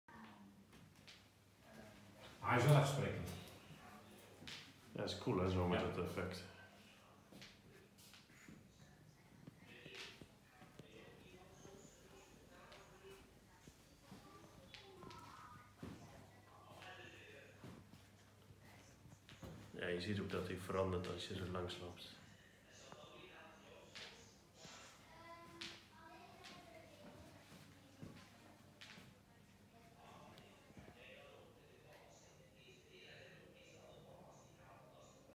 Reginald Art by Reginald Kruger | Personal delivery artwork with Lamborghini Aventador SVJ
See the special light effect of my artwork 'Aventador!' at the delivery of this artwork to the home of the customer. This artwork is tailor made and not for sale.